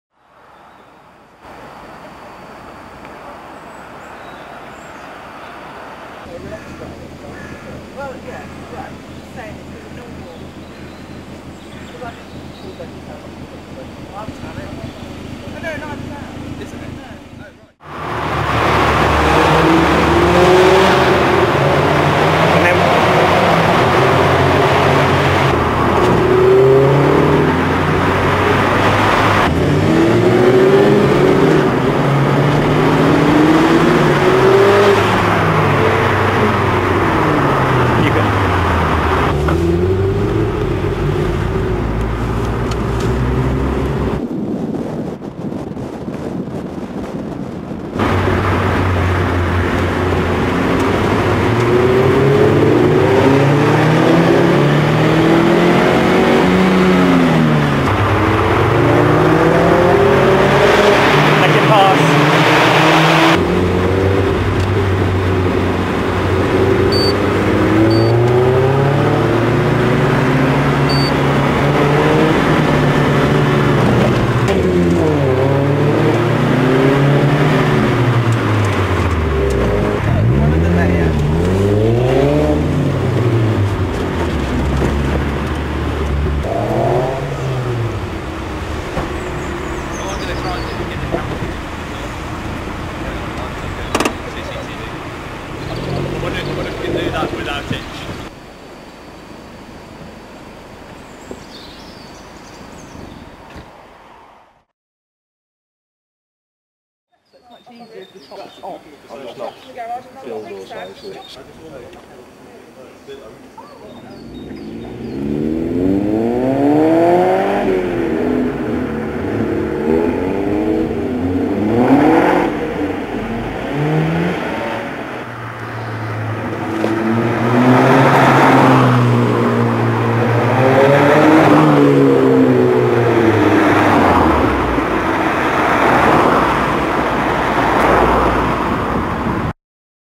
Brummie tunnel run
Vauxhall VX220s driving through Birmingham underpasses to appreciate the sound of the 'sport' exhausts.  8.6MB